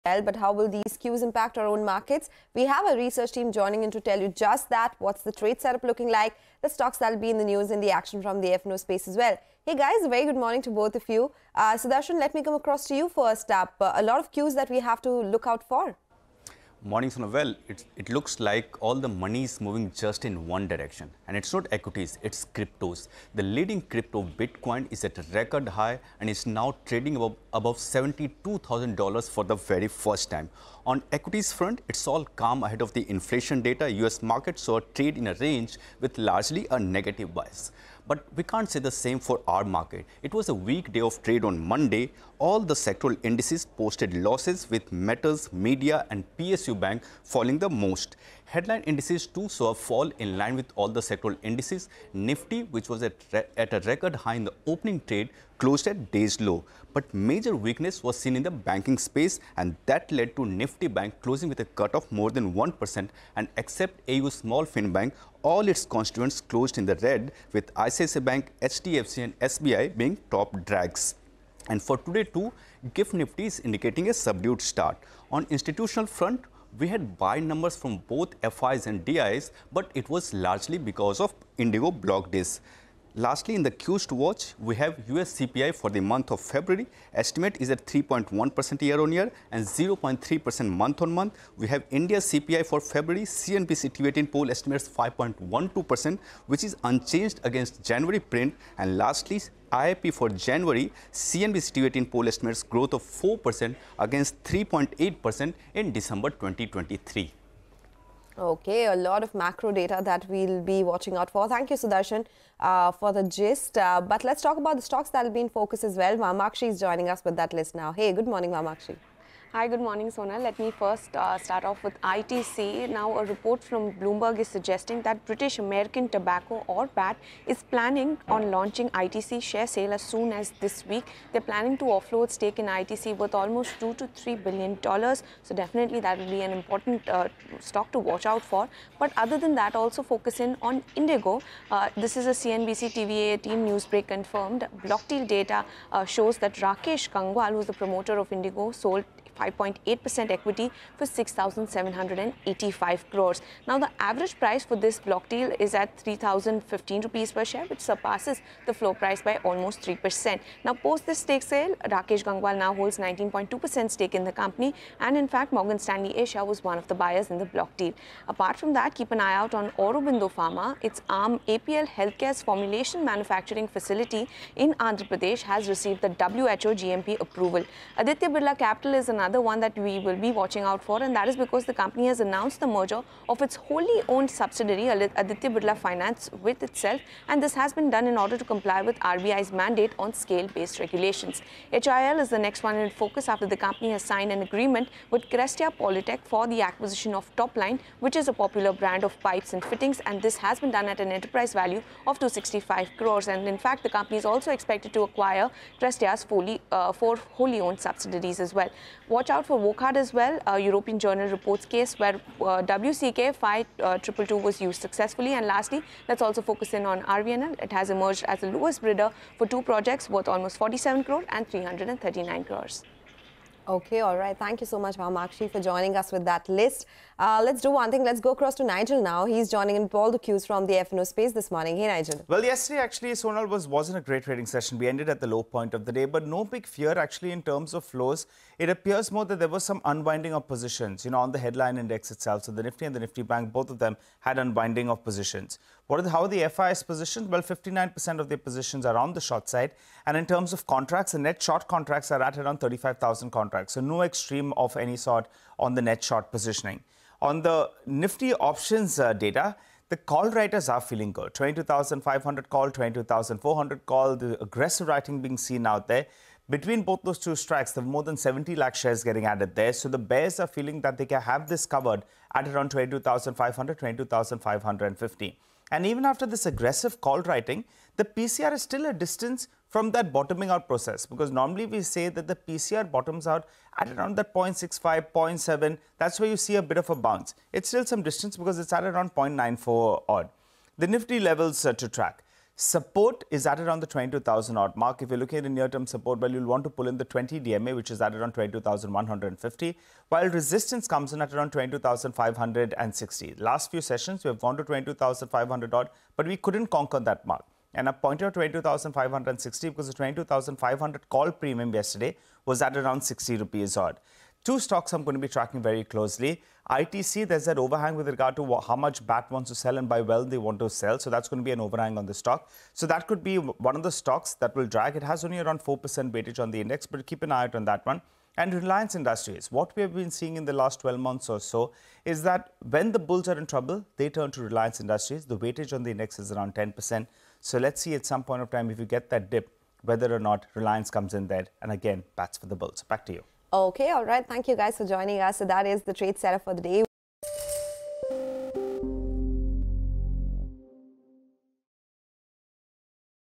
Indian Market To Open On A Flat Note Amid Mixed Global Cues, Indicates GIFT Nifty | CNBC TV18
GIFT Nifty traded marginally higher as the index was up 10.00 points or 0.04% at 22,423.50 indicating a subdued start for domestic indices NSE Nifty 50 and BSE Sensex today. Previously, on Monday, the NSE Nifty 50 lost 160.90 points or 0.72% to settle at 22,332.65, while the BSE Sensex closed 616.75 points or 0.83% lower at 73,502.64.

n18oc_business